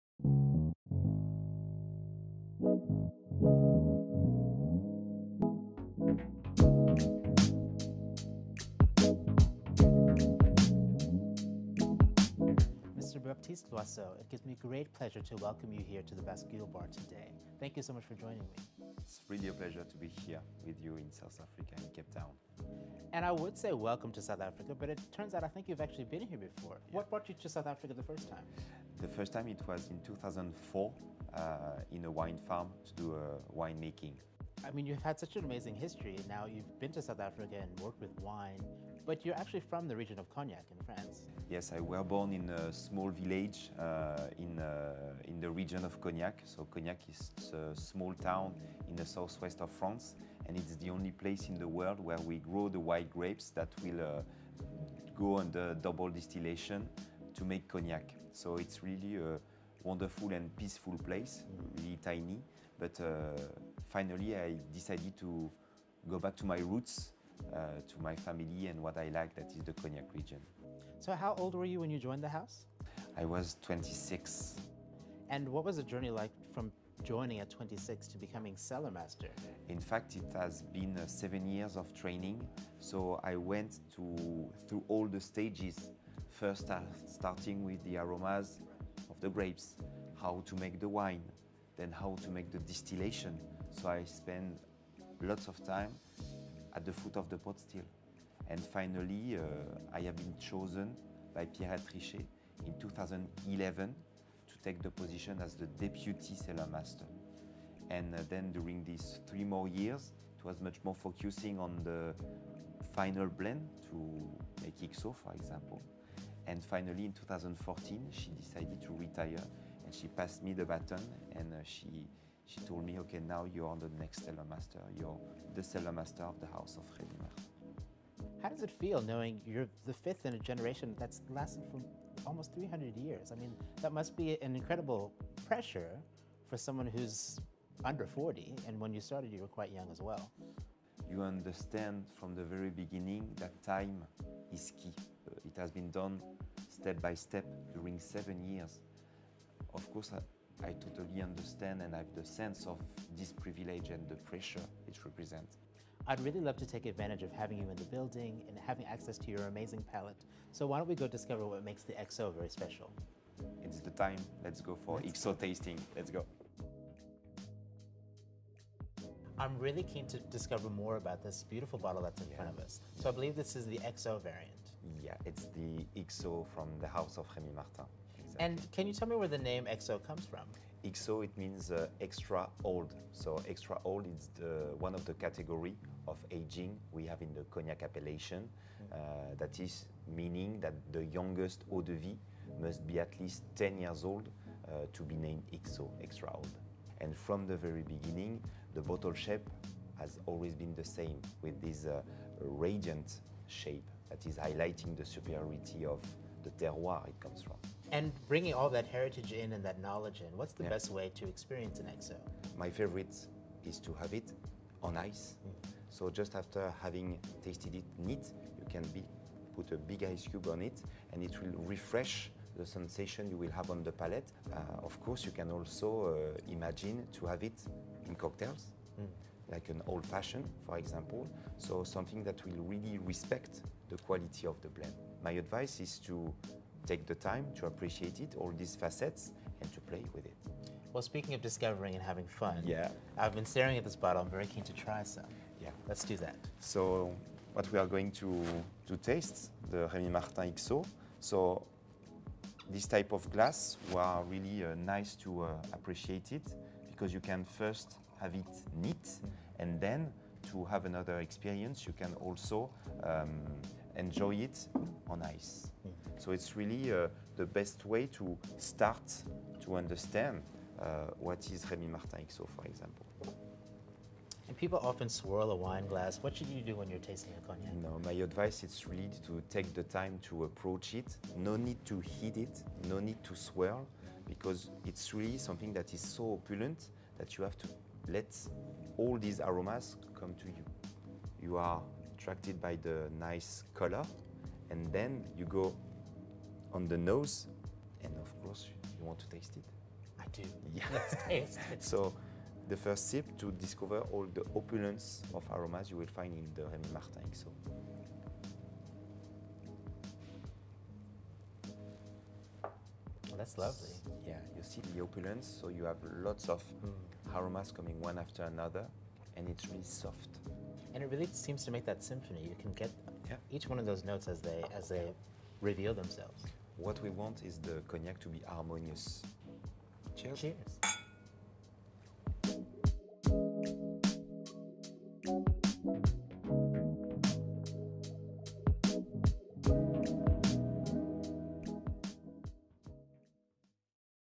In Conversation with Rémy Martin Cellar Master (Baptiste Loiseau ) | Cognac Tasting
Watch as our Editor In Chief Nkosiyati Khumalo sits down with Rémy Martin Cellar Master, Baptiste Loiseau.